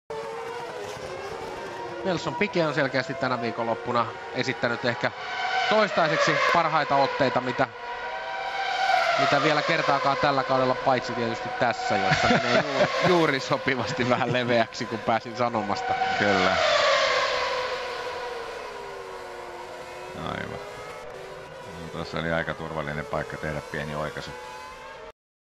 Nelson Piquet - Barcelona 2008
Nelson Piquet cuts the chicane during the FP3 at Barcelona in 2008
Finnish commentary by Timo Pulkkinen & Erkki Mustakari